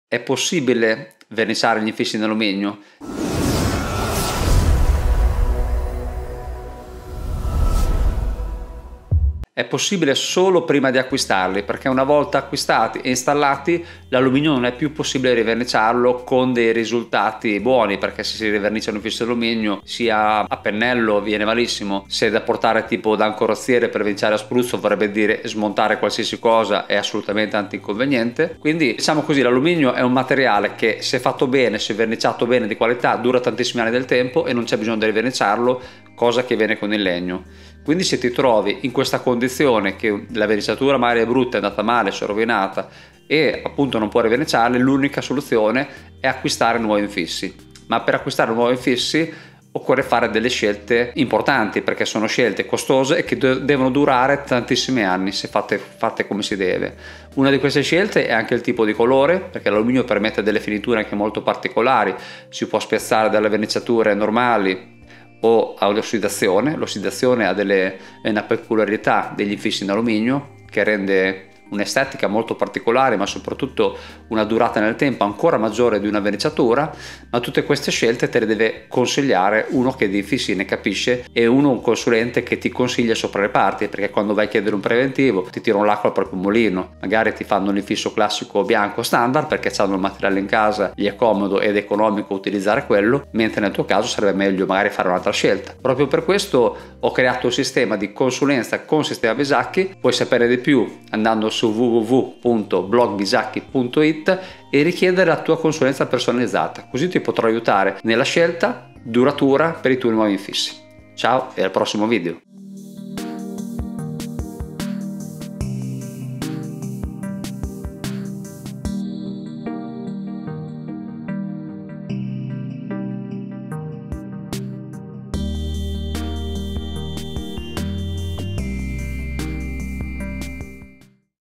verniciare infissi in alluminio
verniciare infissi in alluminio è possibile ?

È possibile solo prima di acquistarli, perché una volta acquistati, e installati, l'alluminio non è più possibile riverniciarlo con dei risultati buoni, perché se si rivernicia un infisso in alluminio, a pennello, viene malissimo. Se è da portare da un carrozziere, per verniciare a spruzzo, vorrebbe dire smontare qualsiasi cosa, è assolutamente anti-conveniente. Quindi, diciamo così, l'alluminio è un materiale che, se fatto bene, se verniciato bene, di qualità, dura tantissimi anni nel tempo, e non c'è bisogno di riverniciarlo, cosa che avviene con il legno. Quindi, se ti trovi in questa condizione, che la verniciatura magari è brutta, è andata male, si è rovinata, e non puoi riverniciare, l'unica soluzione è acquistare nuovi infissi. Ma, per acquistare nuovi fissi, occorre fare delle scelte importanti, perché sono scelte costose, che devono durare tantissimi anni, se fatte come si deve. Una di queste scelte è anche il tipo di colore, perché l'alluminio permette delle finiture anche molto particolari, si può spaziare dalla verniciatura normali, o ad ossidazione. L'ossidazione è una peculiarità degli infissi in alluminio, che rende un'estetica molto particolare, ma soprattutto una durata nel tempo ancora maggiore di una verniciatura. Tutte queste scelte le deve consigliare uno che di infissi ne capisce, e un consulente che ti consiglia sopra le parti perché, quando vai a chiedere un preventivo, ti tirano l'acqua al proprio mulino; magari ti fanno un infisso classico, bianco, standard, perché c'hanno il materiale in casa, gli è comodo, ed è economico utilizzare quello, mentre nel tuo caso sarebbe meglio magari fare un'altra scelta. Proprio per questo ho creato il sistema di consulenza con sistema Bisacchi. Puoi sapere di più andando su e richiedere la tua consulenza personalizzata, così ti potrò aiutare nella scelta duratura per i tuoi nuovi infissi. Ciao e al prossimo video.